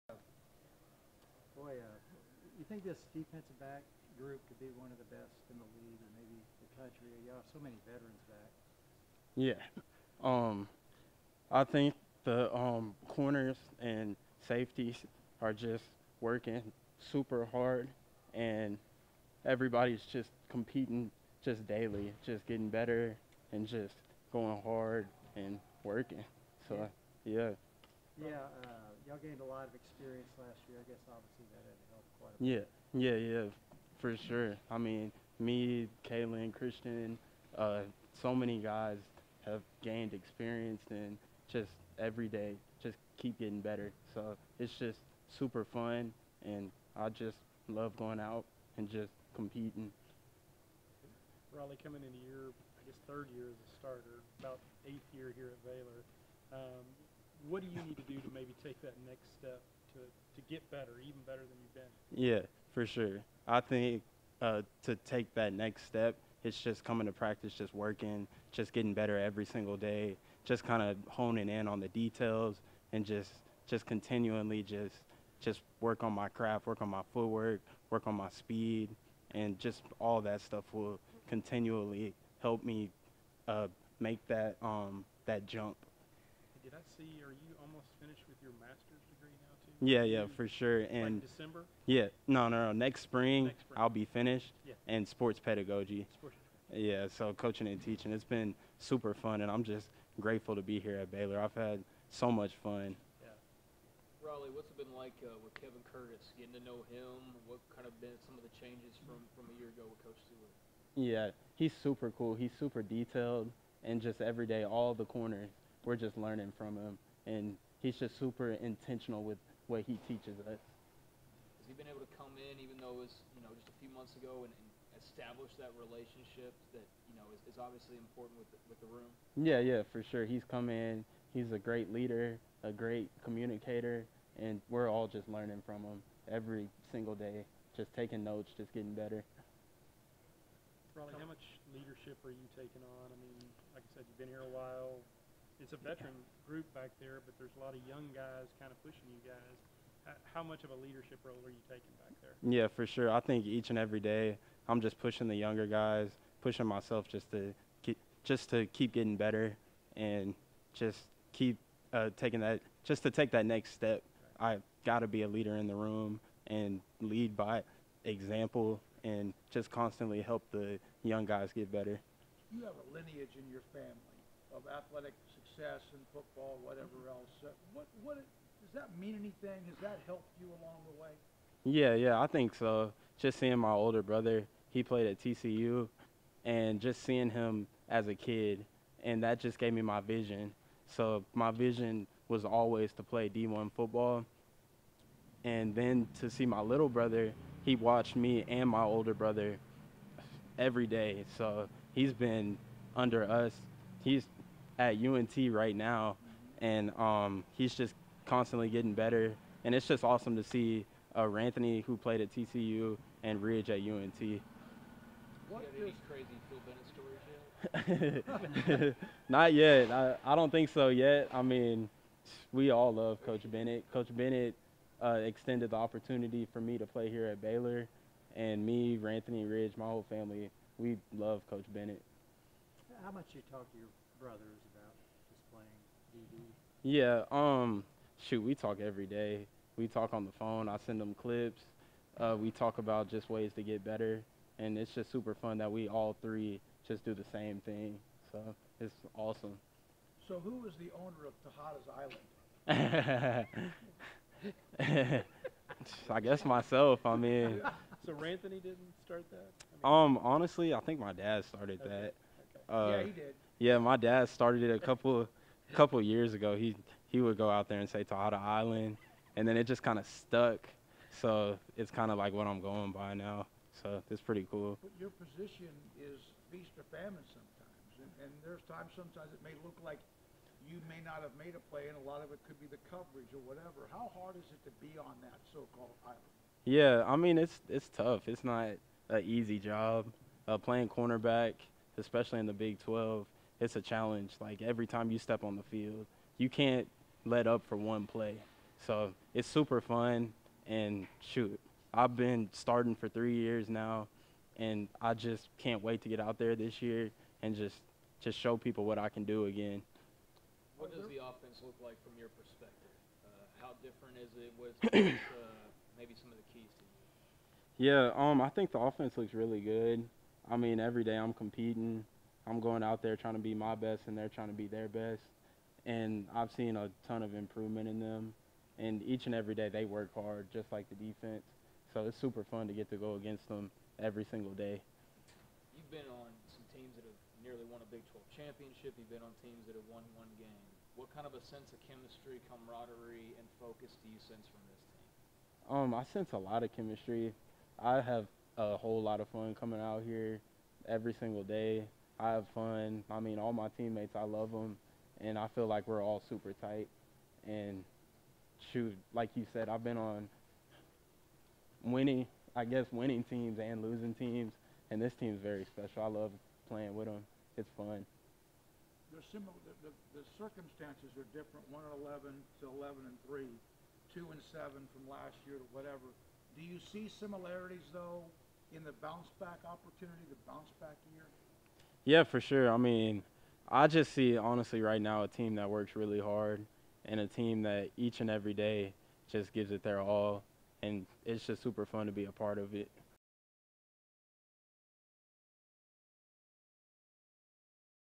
Baylor Football: Raleigh Texada Media Availability (August 16, 2021)
Senior cornerback Raleigh Texada talks to the media after day 9 of preseason camp.